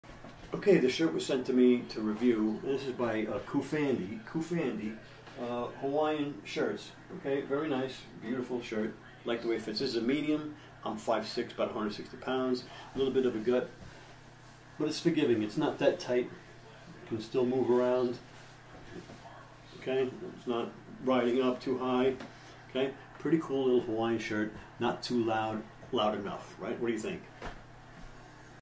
CoofAndy Hawaiian Shirt Flower Yellow
This men’s short sleeve Hawaiian shirt is made of quality fabric, which is lightweight, breathable, and comfortable to wear.
The mens beach shirt features, short sleeve, spread collar, chest pocket and button closure. Using loud colours and bold patterns make our printed shirts the perfect outfits for summer festivals.